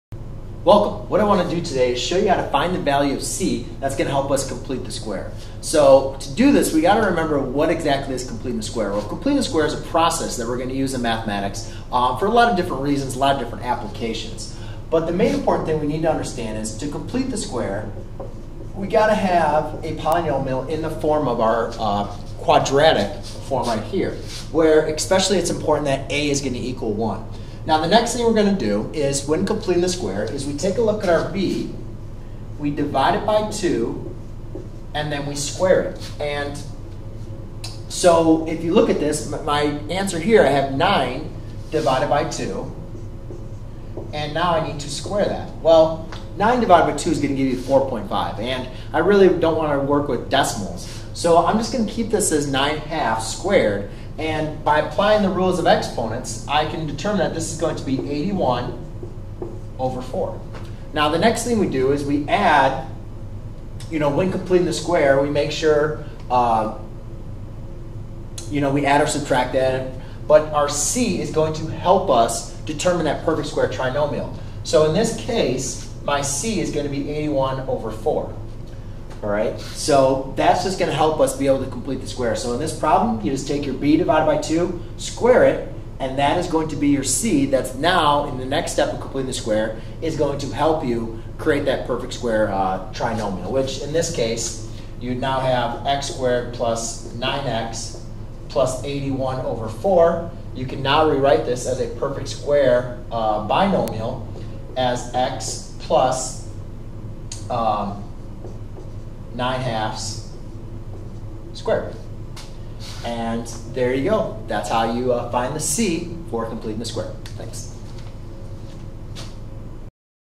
Find the value of c that completes the square
👉 Learn how to find the value c that completes the square in a quadratic expression. A quadratic expression is an expression whose highest exponent in the variable(s) is 2. It is of the form ax^2 + bx + c where a, b, and c are constants. The value c that we create is what will turn our quadratic expression into a perfect square trinomial. A perfect square trinomial is an expression that can be expressed in the form (x + a)^2.

Given a quadratic expression, ax^2 + bx + c, the value of c that completes the square in the quadratic expression is given by c = (b/2)^2.